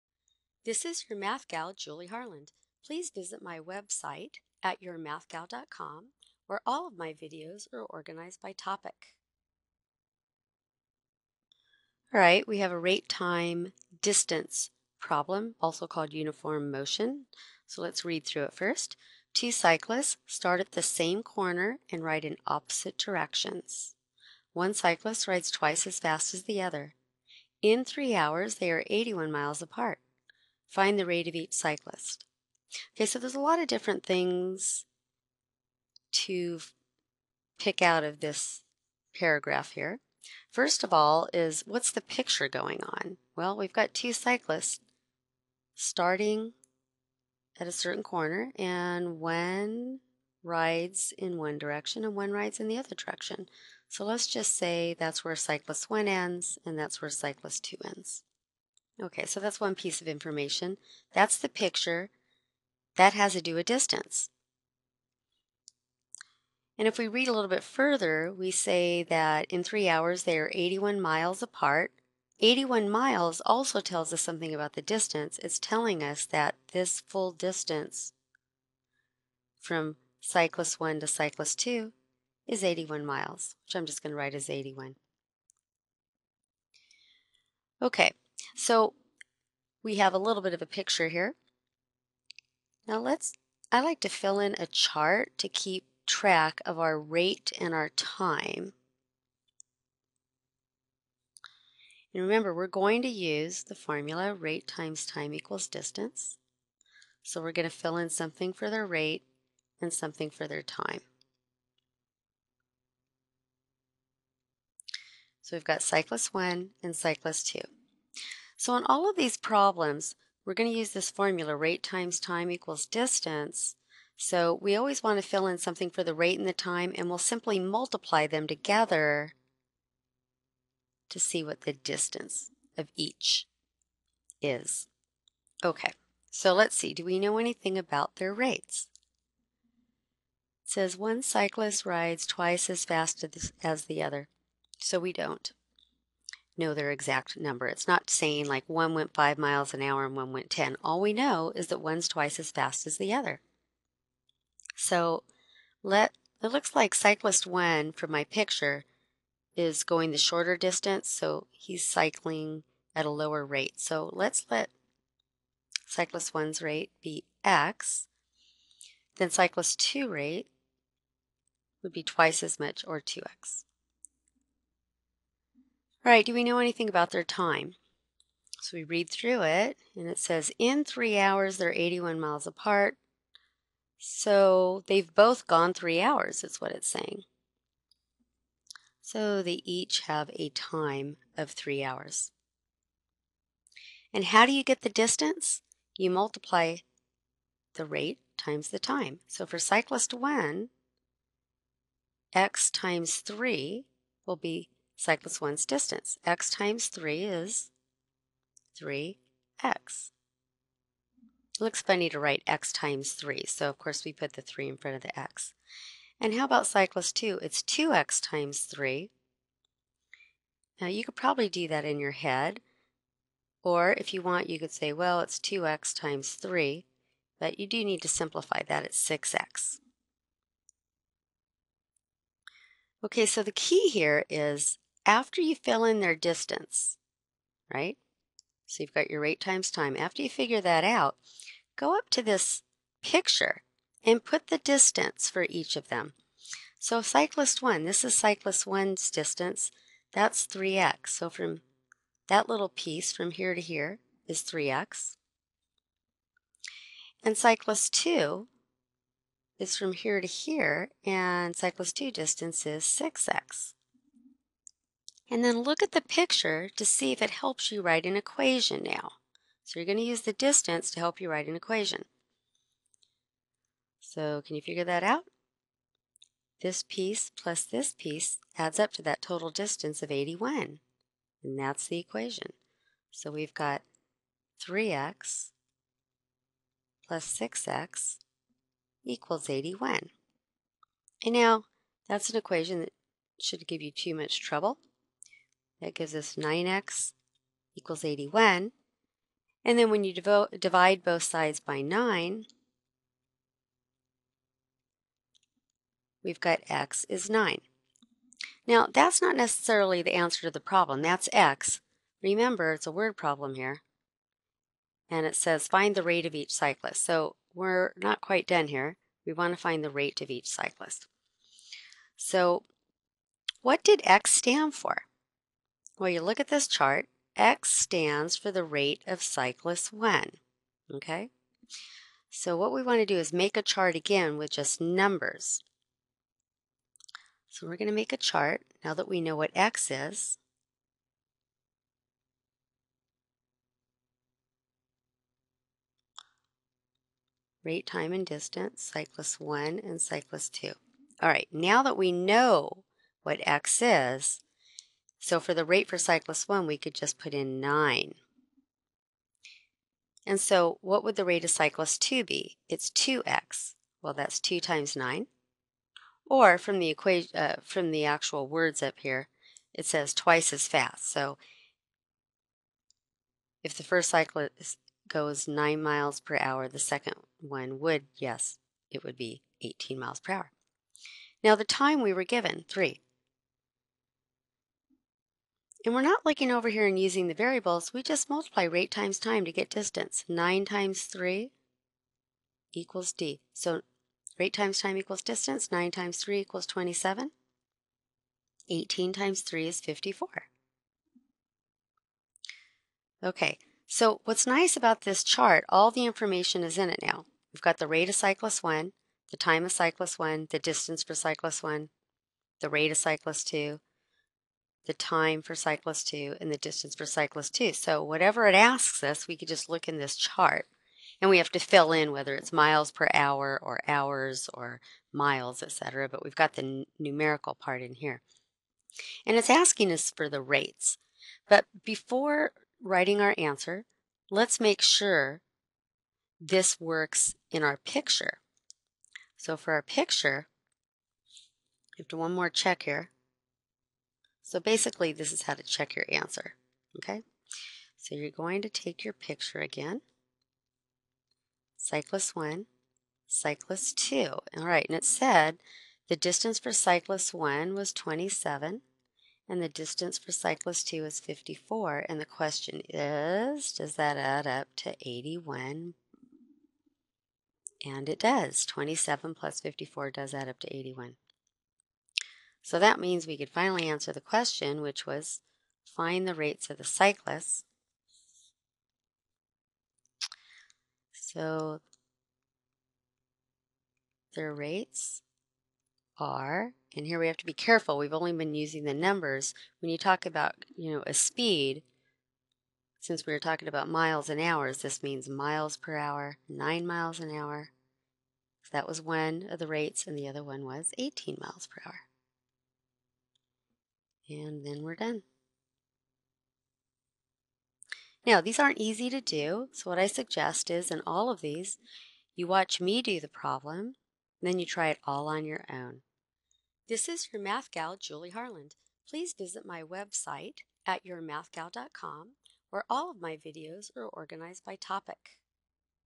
Rate Time Distance 1 new
Videos by Julie Harland organized at YourMathGal.com. Part of a series of examples solving word problems involving uniform motion problems. These problems require basic algebra, and only linear equations in one variable are used.

rtdnew1.mp4